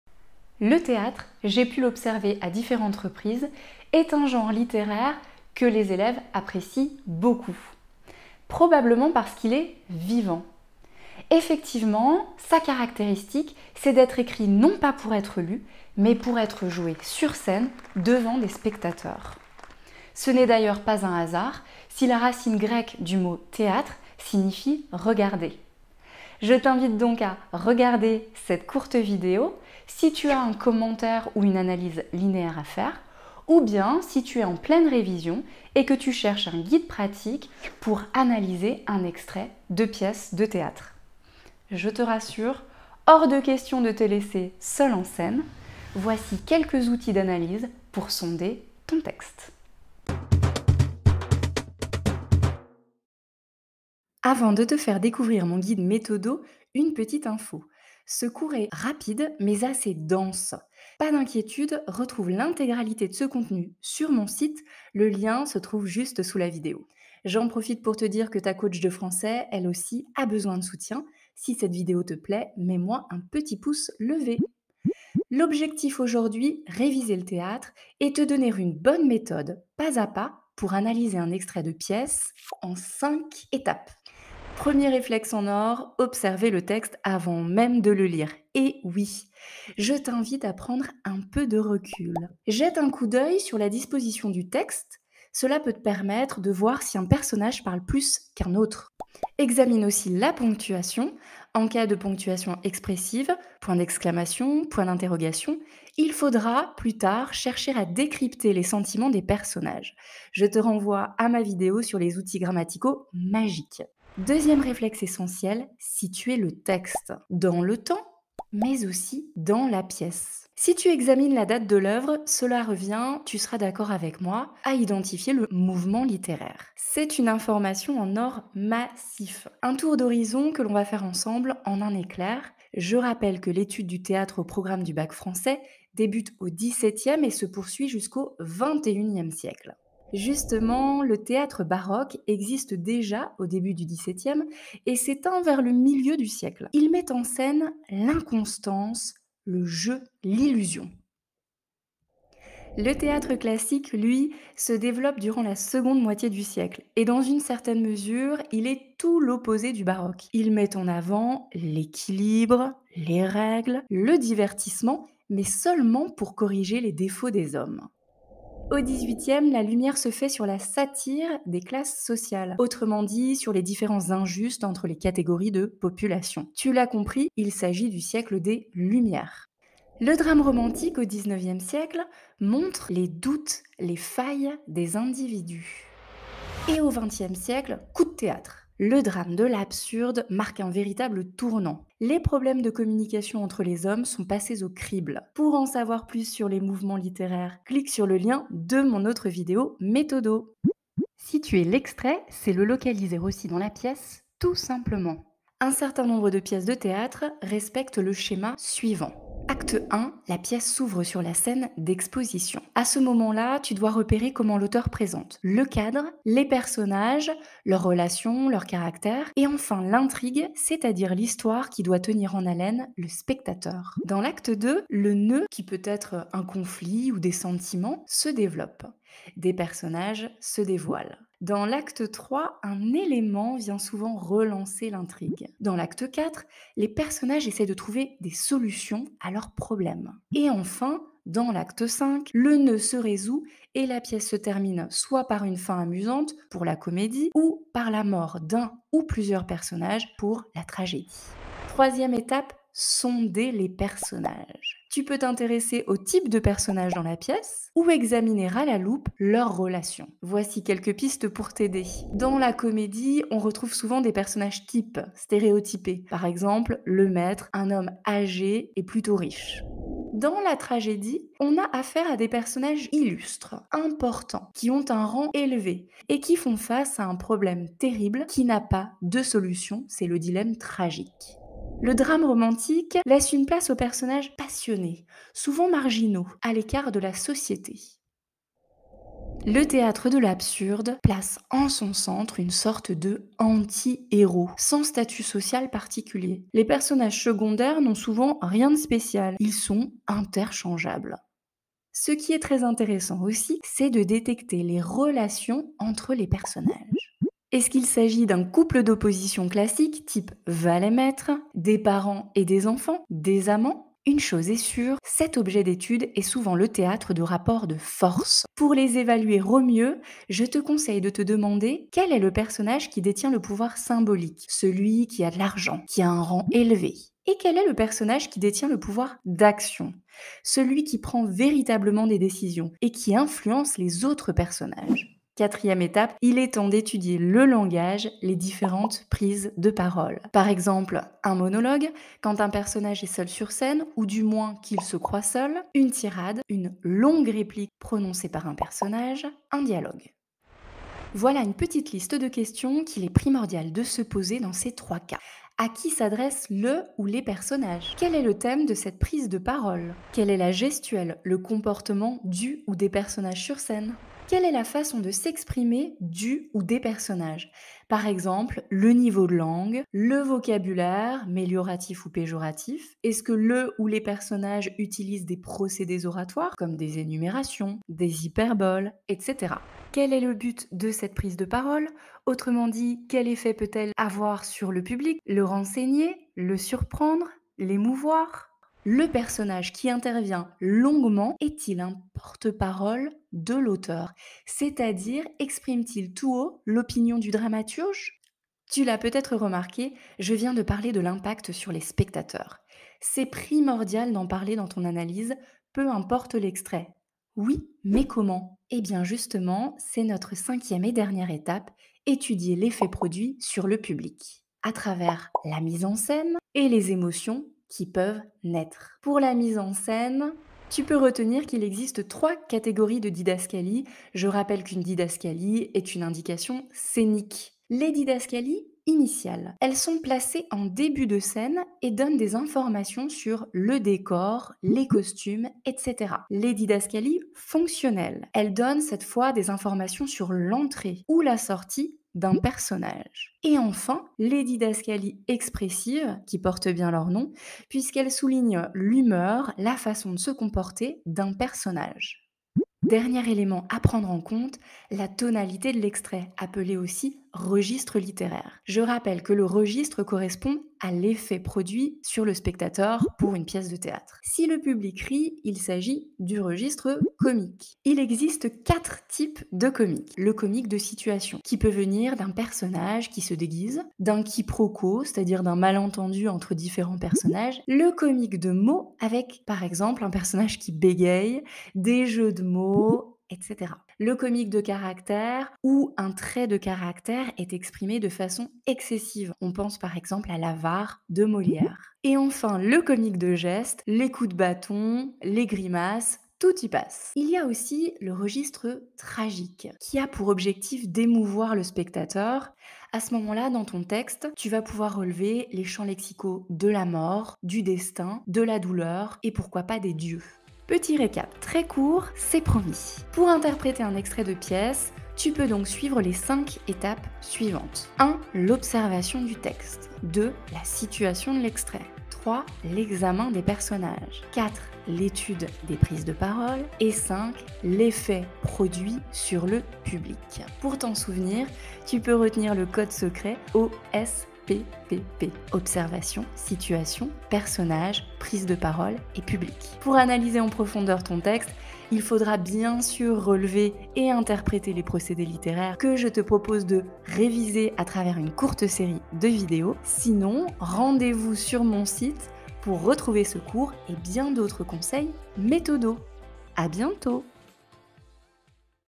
🎭 Analyser une pièce de théâtre |💡 RÉVISION EXPRESS
✅ Tu dois analyser un extrait de pièce de théâtre ? Ou bien réviser le théâtre pour le bac français ? À travers cette courte vidéo, je te donne des outils d'analyse en or massif. Tu n'as plus qu'à te laisser guider !

▬▬▬▬▬▬▬▬▬▬▬  ⬇️  MES COURS ⬇️ ▬▬▬▬▬▬▬▬▬▬

▬▬▬▬▬▬▬▬▬▬▬  ➕ LES TEMPS FORTS ➕ ▬▬▬▬▬▬▬▬▬▬▬

0:00 Introduction 👋
1:14 Observer ton texte 👀
1:54 Situer l'extrait 🔍
4:11 Sonder les personnages 🤦🏻‍♂️
5:35 Analyser les prises de parole 🗣
6:49 Identifier l'effet sur le public 🎭

▬▬▬▬▬▬▬▬▬▬▬ 🎥 MES VIDÉOS ASSOCIÉES 🎥 ▬▬▬▬▬▬▬▬▬▬▬

Olivia, ta coach de français 💪🏻 💪🏻